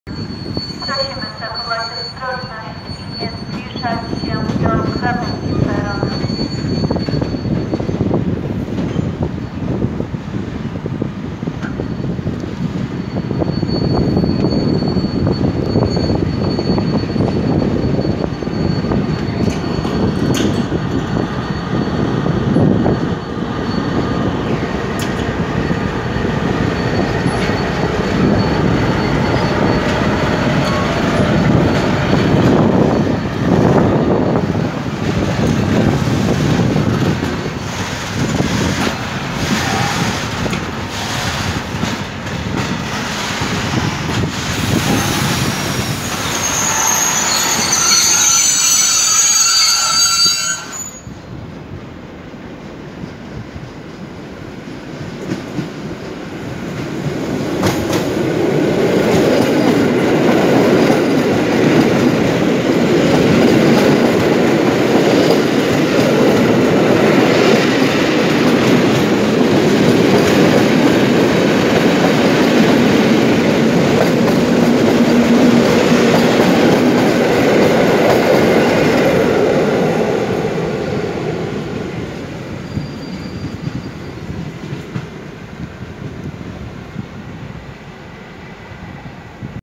Trzebinia - Podwójna mijanka BR232-534 | EN57-2051 & EN57-2061 | ED72-011
- Trzebinia -
0:00 | CTL Logistics - BR232-534 ze składem węglarek
0:54 | POLREGIO - R |43412/3| EN57-2051 & EN57-2061 relacji Oświęcim - Kraków Główny
1:10 | POLREGIO - R |34403/5| ED72-011 relacji Kraków Główny - Czechowice-Dziedzice

Data nagrania: lipiec 2019 r.